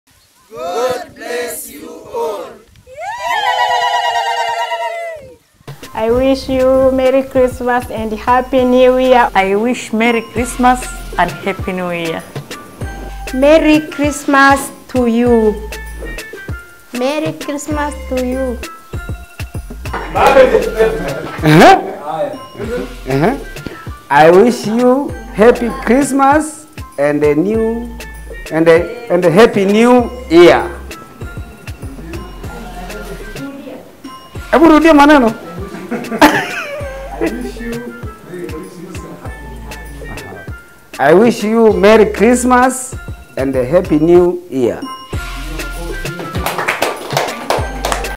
Merry Christmas from Food for His Children!
We are so grateful to all the incredibly generous people who give all they can so that Food for His Children Families can become all God created them to be and break the chains of generational poverty, unleasing their potential. Merry Christmas and Happy New Year!